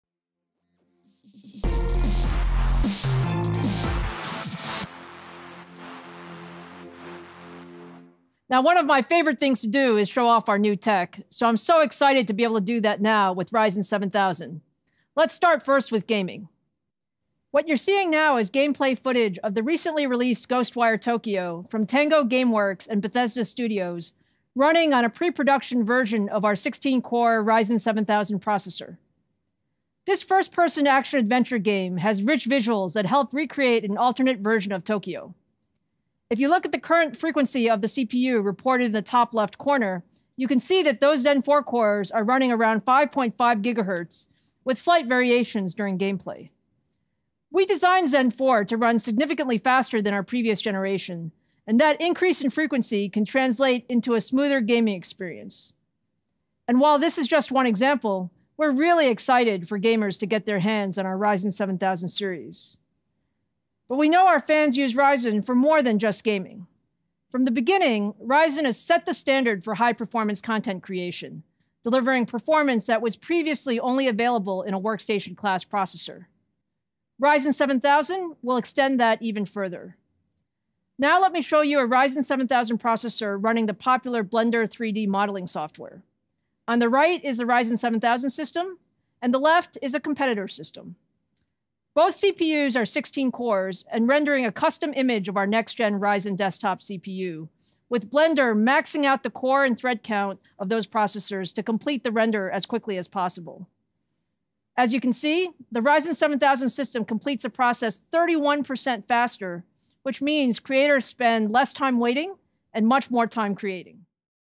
AMD Computex 2022: Ryzen 7000 Performance-Demos
Auf der Keynote der Computex 2022 präsentierte AMD einen Gaming- und einen Workstation-Benchmark zu den neuen Ryzen-Prozessoren auf Basis von Zen 4.